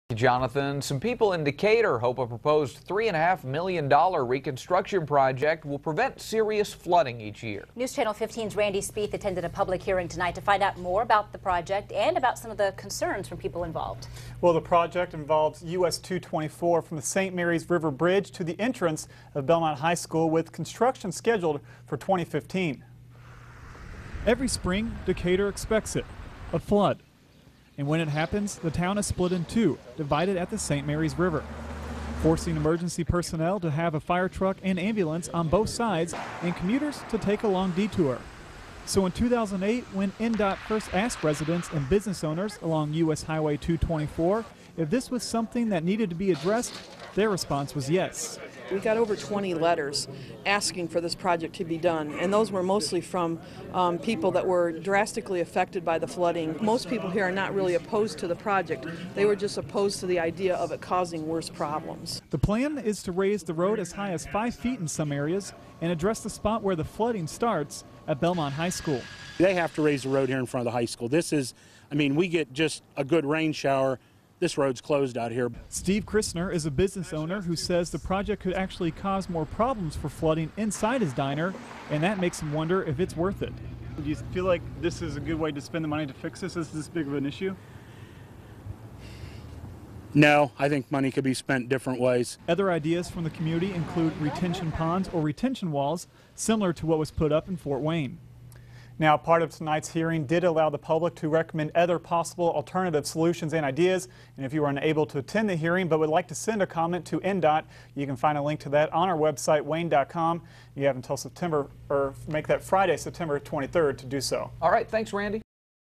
INDOT holds public hearing in Decatur
The Indiana Department of Transportation held a public hearing in Decatur on Tuesday night, to get the public's input on a $3.5 million construction project.